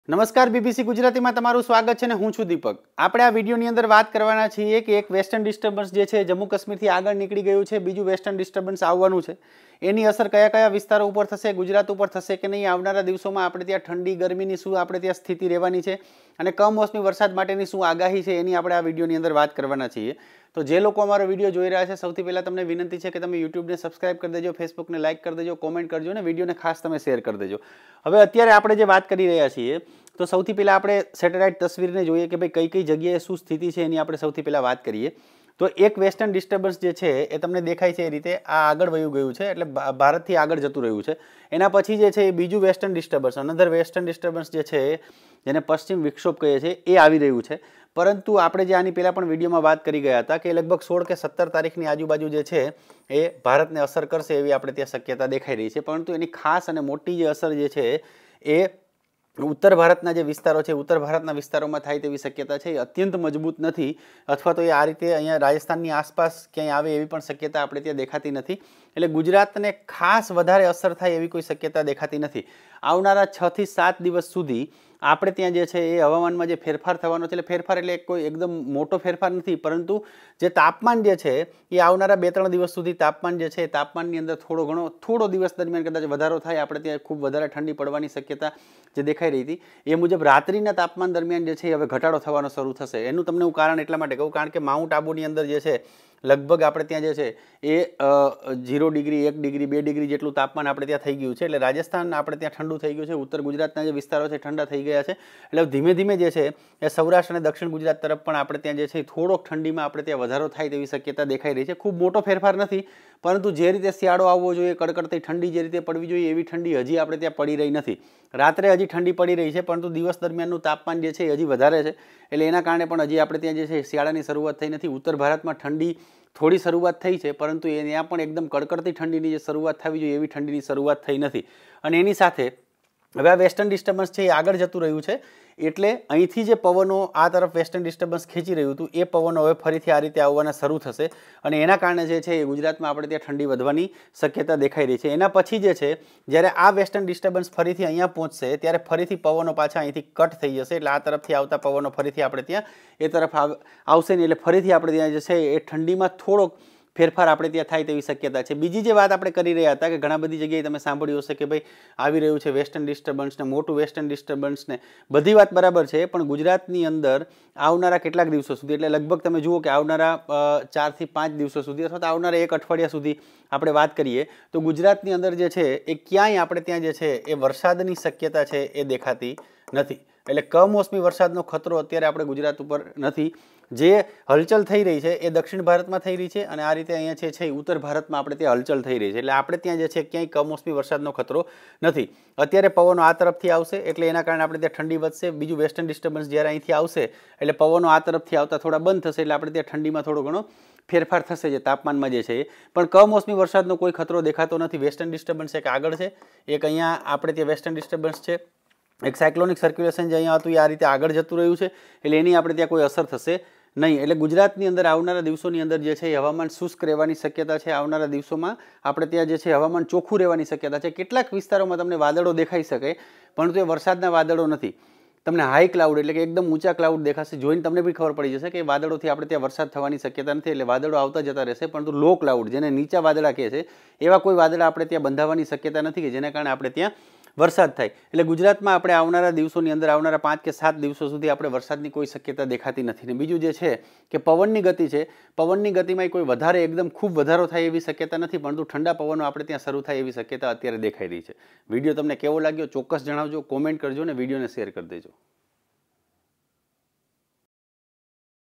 Gujarat Weather : રાજયમાં કડકડતી ઠંડી પડવાની કયારથી શરૂ થશે?
ગુજરાતમાં કમોસમી વરસાદ બાદ હવે સવાલ એ થઈ રહ્યો છે કે ઠંડી ક્યારથી પડશે? અરબી સમુદ્રમાં સિસ્ટમ કઈ તરફ આગળ વધશે એ જોવું પણ મહત્ત્વનું છે, એની ગુજરાત પર કેવી અસર થશે. સાથે જ ગુજરાતમાં ઠંડી વધવાની શરૂઆત કયારથી થશે? ગુજરાતના હવામાન વિશેની વિગતવાર માહિતી સ્ક્રીન પરના નકશાઓની મદદથી સમજાવીશું આ વીડિયોમાં.
અહેવાલ અને રજૂઆત -દીપક ચુડાસમા, ઍડિટ - સુમિત વૈદ

તમે અમારી સાથે જોડાઈ શકો છો :

JioChat Channel : BBC Gujarati
ShareChat : bbcnewsgujarati